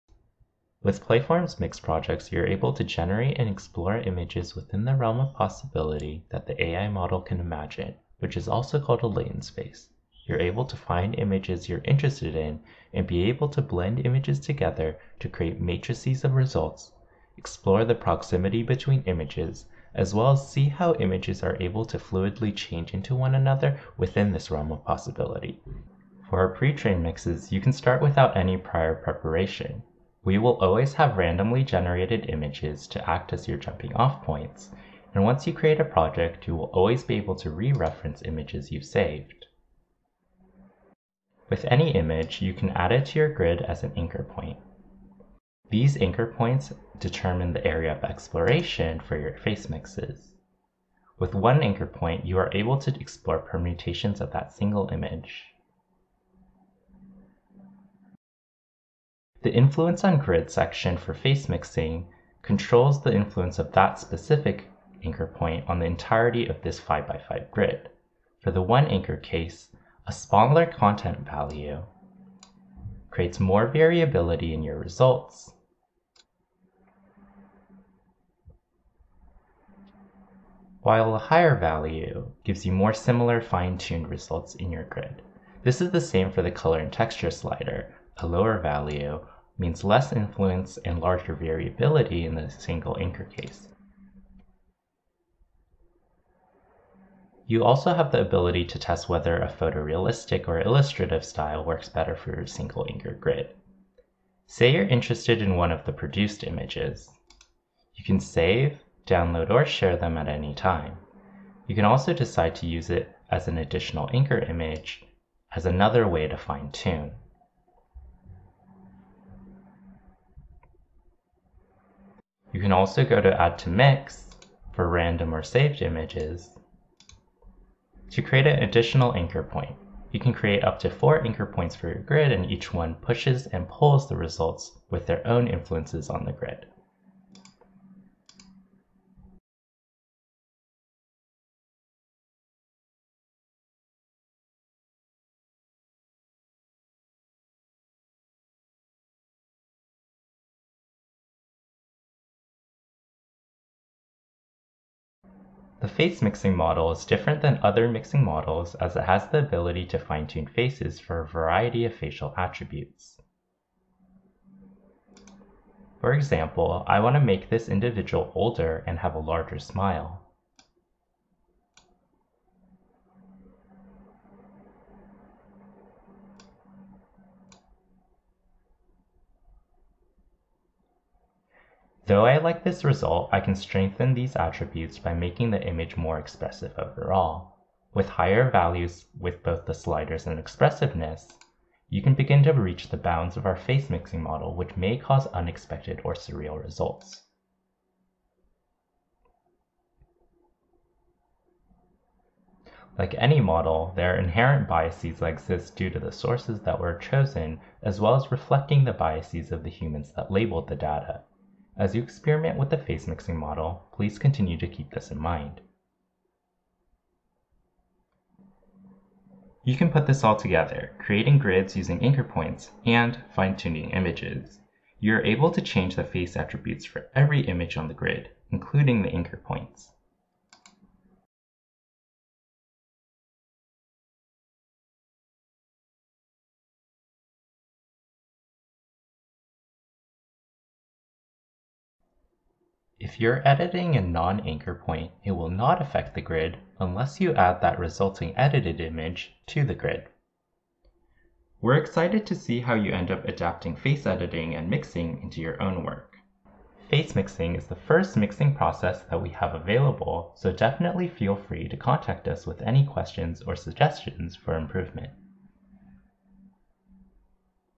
Playform FaceMix Tutorial
How to use AI to generate and manipulate faces using Playform.  Find faces you're interested in, mix together to create new ones with ultimate fine control. Mix photorealistic and/or illustrative faces and edit face attributes to create any desired effect. Attributes you can control includes age, gender, attractiveness, mouth shape, hair color and style, facial hair, and many others.